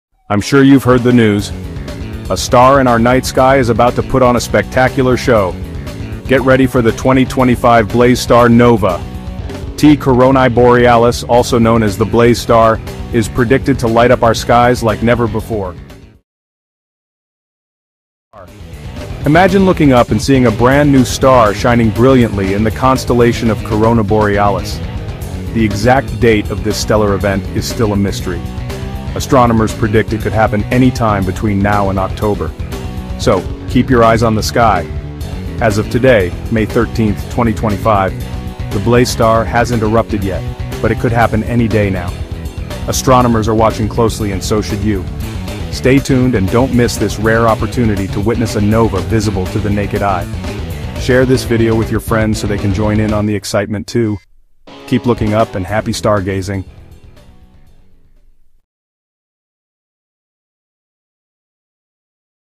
Exploding Star in 2025? Visible to the Naked Eye! 🌟 Let's talk about T coronae Borealis !
A cosmic light show is coming! Astronomers have predicted that a rare stellar explosion—a nova—is expected to occur in a binary star system known as T Coronae Borealis (the "Blaze Star") sometime between now and September 2025. Located about 3,000 light-years from Earth in the constellation Corona Borealis, this system erupts roughly every 80 years.

When it goes nova, its brightness will increase dramatically and may reach a magnitude of 2 or even brighter—making it easily visible to the naked eye without a telescope for up to a week! Imagine seeing a “new star” suddenly appear in the sky. This could be your once-in-a-lifetime chance to witness a live stellar eruption from your own backyard.

NASA and observatories worldwide are closely monitoring the system. Keep your eyes on the skies, especially this summer. Follow for updates on where and when to look!